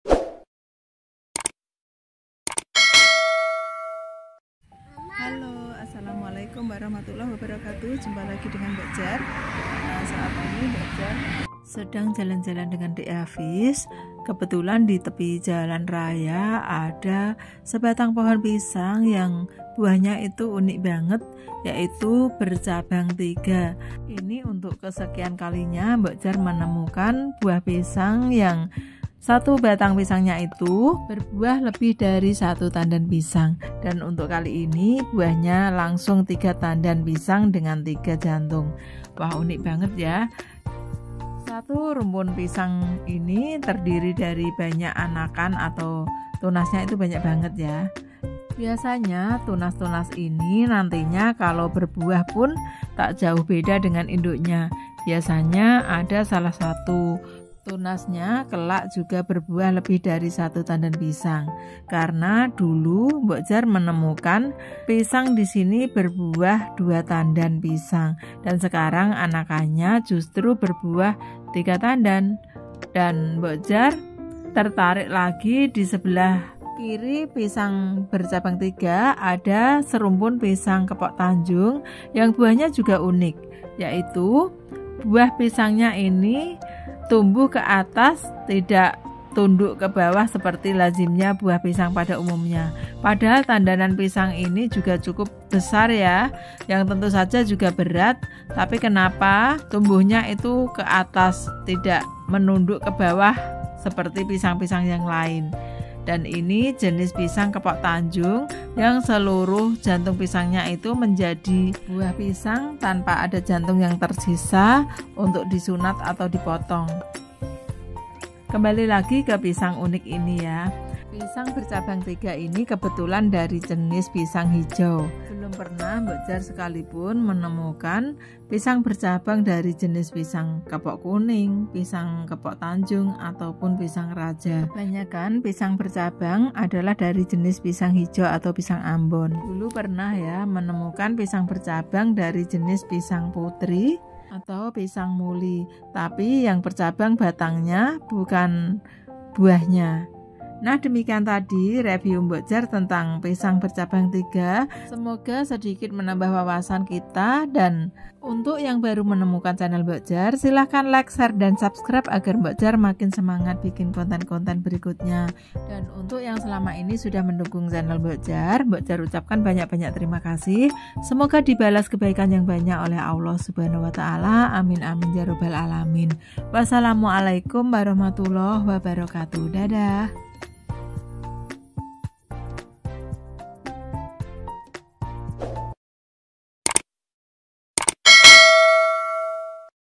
VIRAL!!! PISANG BERCABANG TIGA
Untuk kesekian kalinya mbok Jar menemukan pohon pisang berbuah lebih dari satu tandan pisang.Kali ini buahnya ada tiga tandan sekaligus dan berada di tepi jalan raya TAYU -Juana. pisang dari jenis pisang hijau ini berdampingan dengan pisang kepok tanjung yang buahnya tak kalah unik..
karena tandanannya tegak keatas tidak menunduk kebawah layaknya pisang pada umumnya.